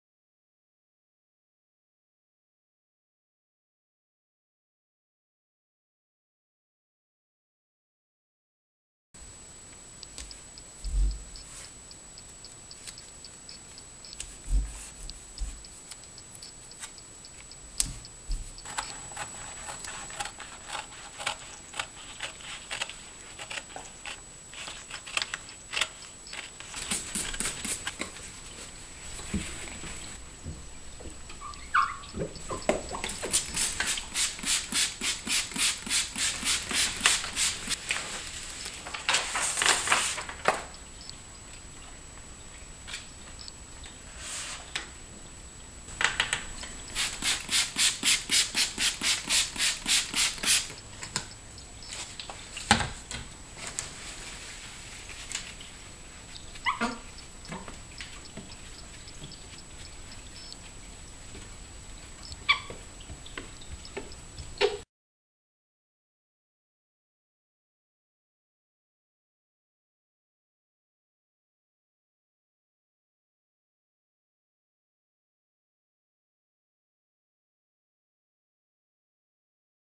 Gila Window Tint - review and Installation
Tinting windows on the sunny side can be a great way to cut cooling costs.  In my last video on the subject I used Energy Film. I discovered this Spring that the tinted film doesn't seem to be on the market anymore.   This time, I bought a similar product by Gila.  It's a cling film.  There is no glue.  It was half the price of the Energy Films and went on easily.  It was darker than what I was after. Otherwise, I am happy with it.